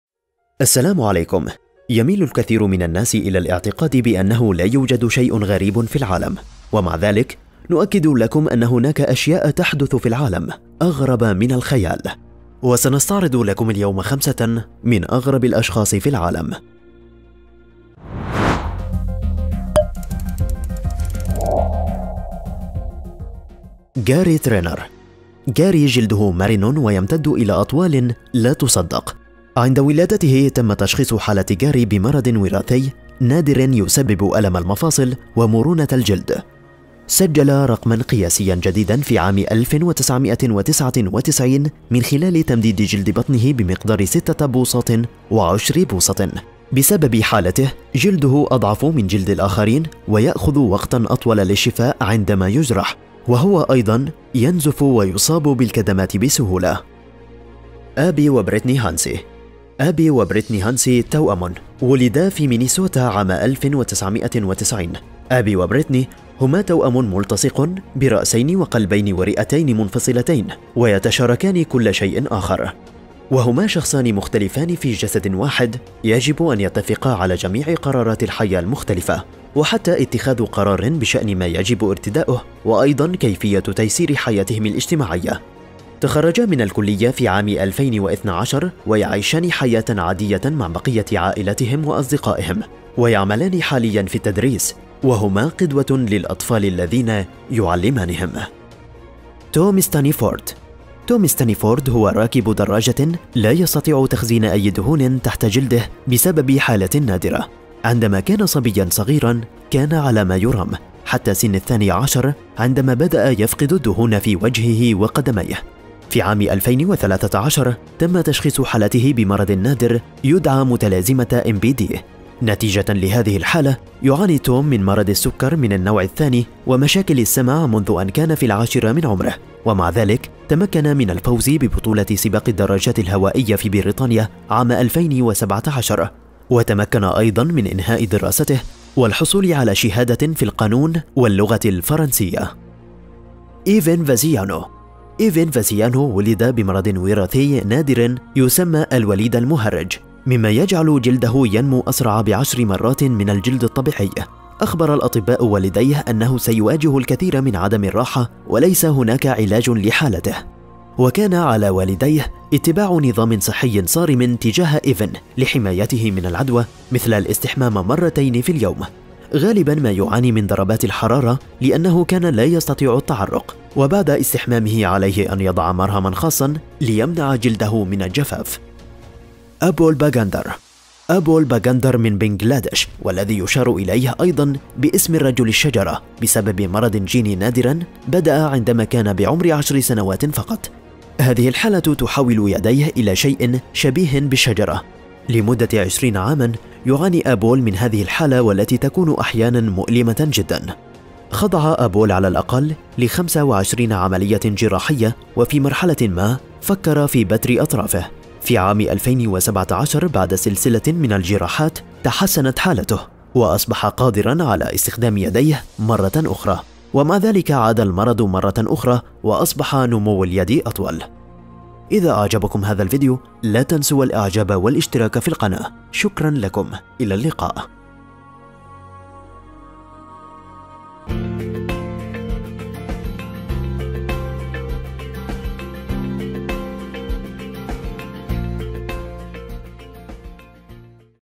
5 من أغرب البشر الموجودين على الارض !!
5 من اغرب الأشخاص حول العالم
يميل الكثير من الناس الى الاعتقاد بأنه لا يوجد شيء غريب في العالم، ومع ذلك نؤكد لكم أن هناك أشياء تحدث في العالم أغرب من الخيال، وسنعرض لكم اليوم 5 من أغرب الأشخاص في العالم
وسع معرفتك وإكتشف العالم مع قناة | ( لمحة )